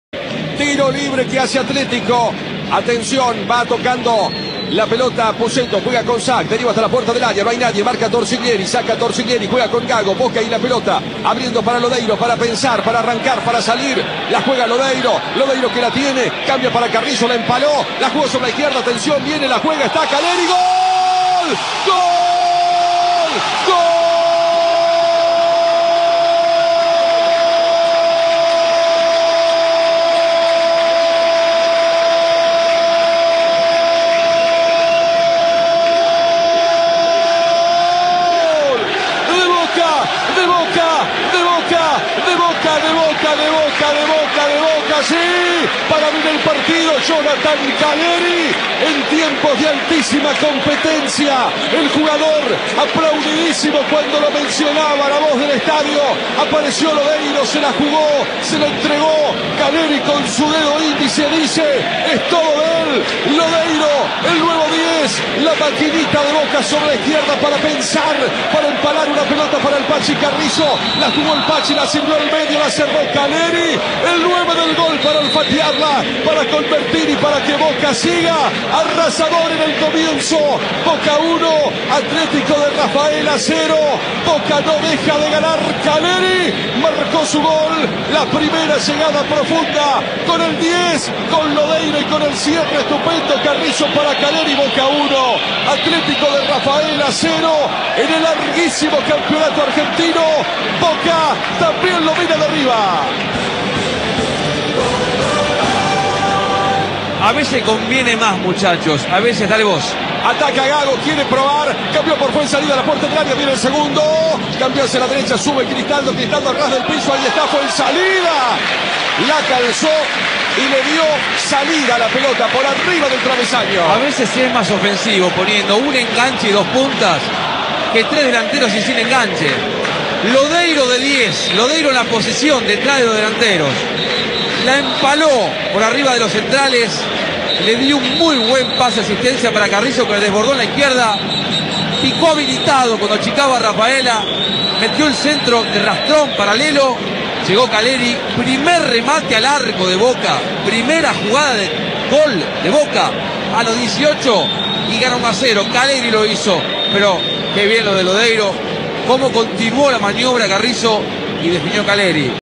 Boca 1 Atletico de Rafaela 0 (Relato Paulo Vilouta) Torneo Primera Division 2015
El gol del partido con relato de Paulo Vilouta Am 910 Radio La Red